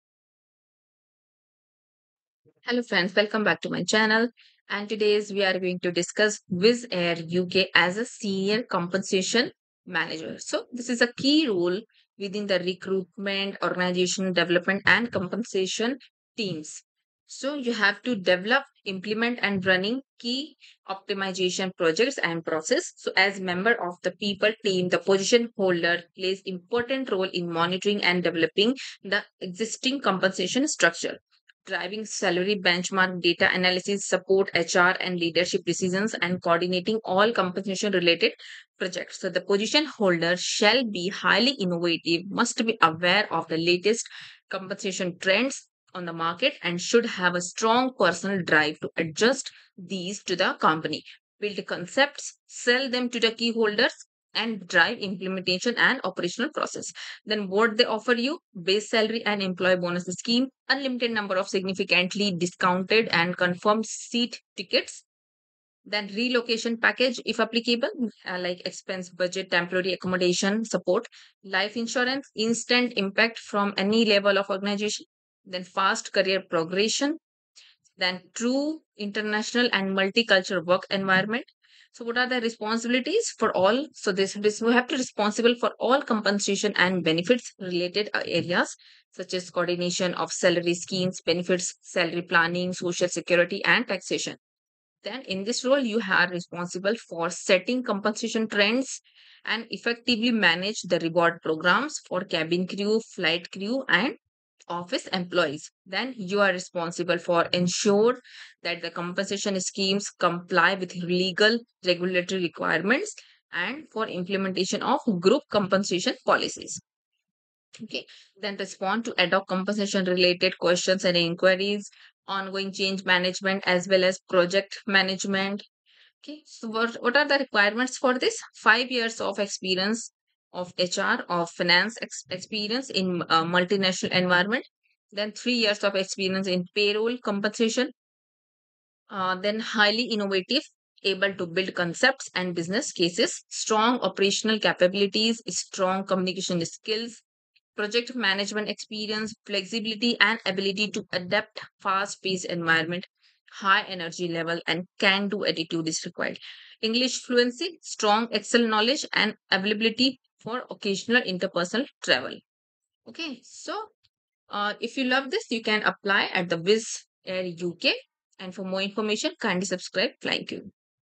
Openings in Wizz Air UK Senior Compensation Manager
Wizz Air UK Senior Compensation Manager

This is a key role within the Recruitment, Organization Development and Compensation teams owning, developing, implementing and running key compensation projects and processes. As member of the People team the position holder plays important role in monitoring and developing the existing compensation structure, driving salary benchmark data analysis, supporting HR and leadership decisions and coordinating all compensation related projects. The position holder shall be highly innovative, must be aware of the latest Compensation trends on the market and should have a strong personal drive to adjust these to the Company, build concepts, ‘sell’ them to the key stakeholders and drive implementation and operational processes.

What we offer:

Base salary and employee bonus scheme

Unlimited number of significantly discounted and confirmed seating tickets, giving access to a continuously growing network of destinations

Relocation package (if applicable) – expense budget, Temporary accommodation, Administration support

Life insurance

AYCM sport pass contracting possibility

Instant impact, from any level of the organisation, exposure to C-suite management

Fast career progression, internal mobility and talent management supported by training opportunities (Wizz Academy and bespoke trainings)

True international and multi-cultural work environment, passionate, driven and diverse teams